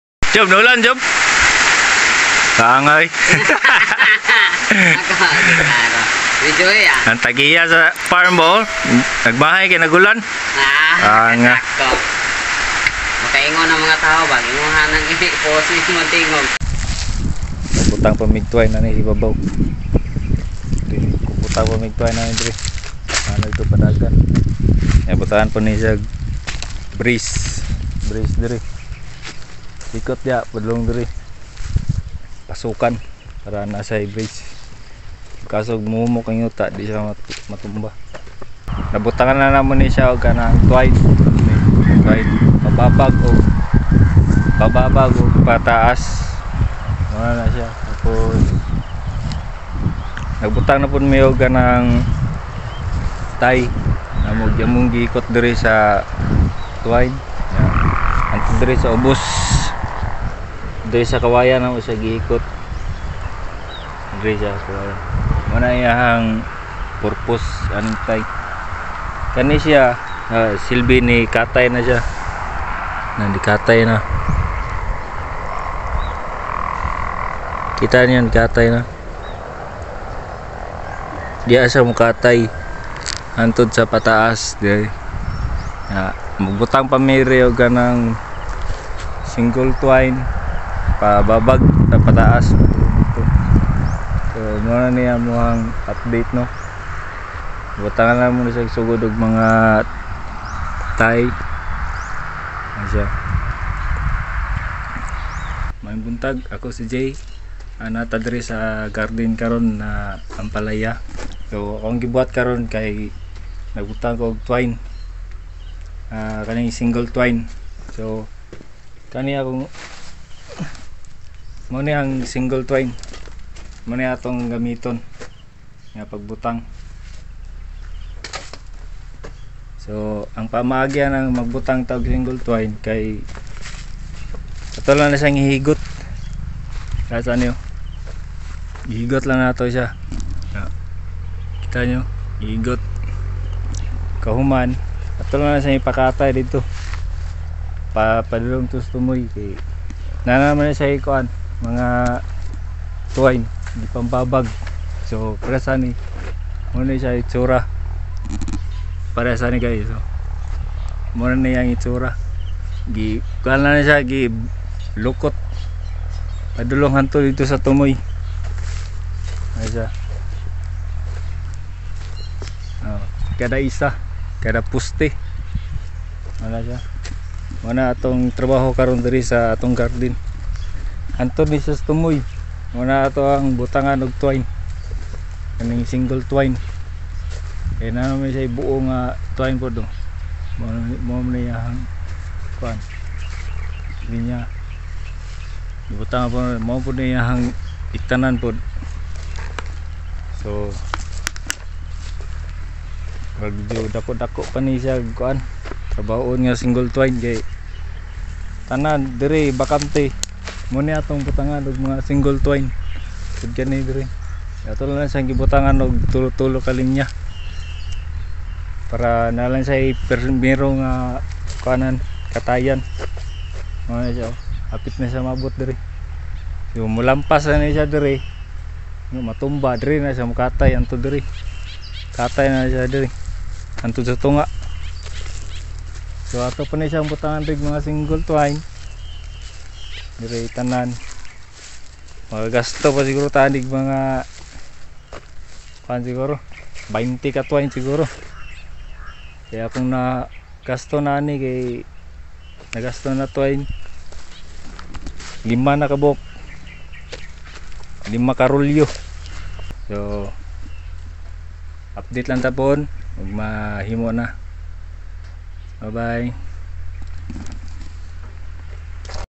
TWINE INSTALLATION|bittergourd
Hi ako si jay sa ating video ngayon ay naglagay tayo ng single twine para sa ating ampalaya.

Ang single twine ay nabili natin ng per kilo, mabibili natin ito sa tindahan ng mga agricultural store.

Ang ginawa ko dito ay naglagay lang ako ng tig tatlo na linya bawat gilid hanggang sa maglagyan lahat.
At naglagay na din ako ng twine na nakatali sa ibaba na siya namang pagkapitan ng ating tanim na ampalaya, paakyat sa taas.

Ang ating tanim na ampalaya ngayon ay 35days old na ngayon.